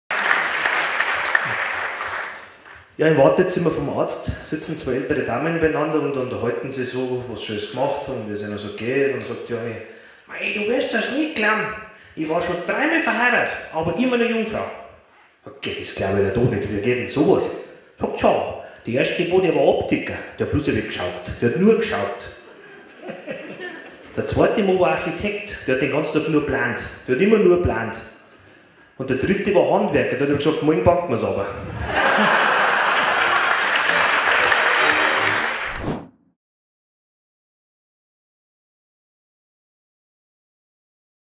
Oberbayerischer Witzemeister 2019 - Bayrischer Witz "Ehemänner"
Livemitschnitt von der Oberbayrischen Witzemeisterschaft 2019 in Dachau.
Viel Spaß !
Video: Harald Meier / Witzemeisterschaft